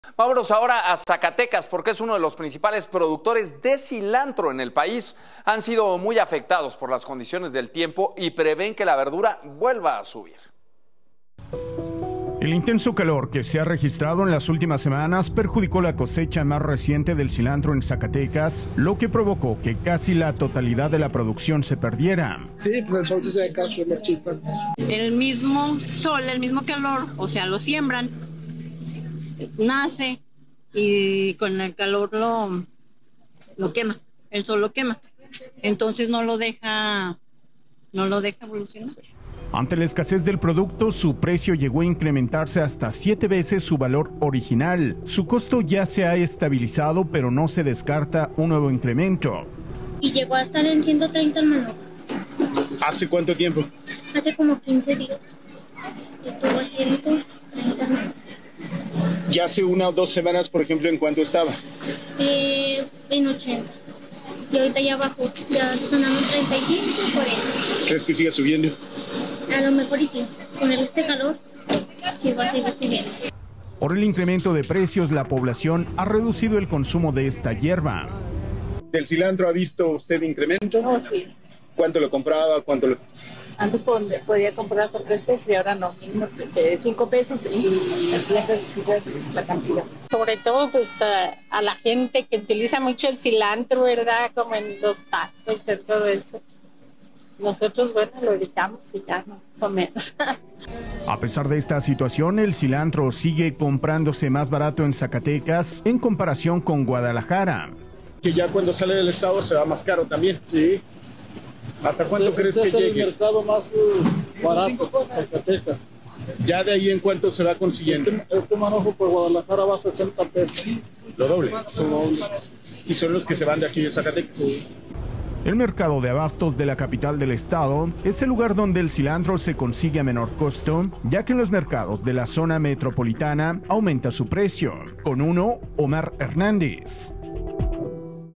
Última cosecha de cilantro quemada, por lo que subiría otra vez el precio.
El intenso calor que se ha registrado en las últimas semanas perjudicó la cosecha más reciente del cilantro en Zacatecas, lo que provocó que casi la totalidad de la producción se perdiera.